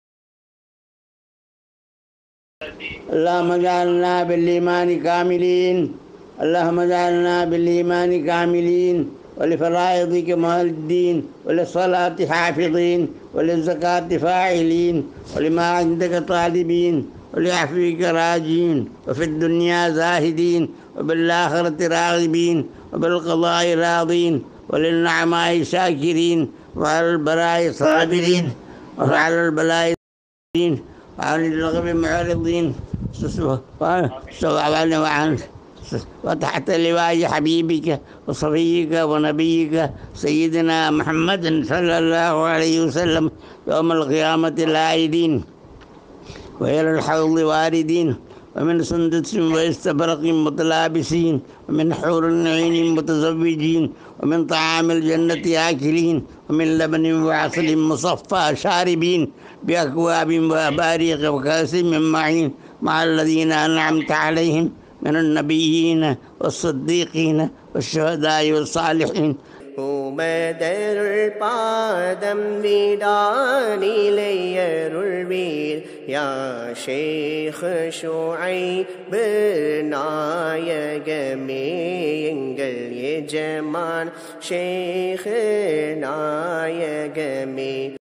Shaikh Nayagam (R.A) dua With translations , ஷேய்கு நாயகம்( ரழி )துஆ மொழிபெயர்ப்புடன்
Excerpts from a dua made by Shaikh  Nayagam Radiyallahu Ta'ala anhu on 17 Ramadan (Apr 2021). With Tamil and English translations..

இந்த வருடம் 2021 ரமழான் மாதம் பிறை 17 அன்று ஷைகு நாயகம் அவர்கள் செய்த துஆக்களில் சில பாகங்கள். (தமிழ் &ஆங்கிலம் மொழிபெயர்ப்புகளுடன்)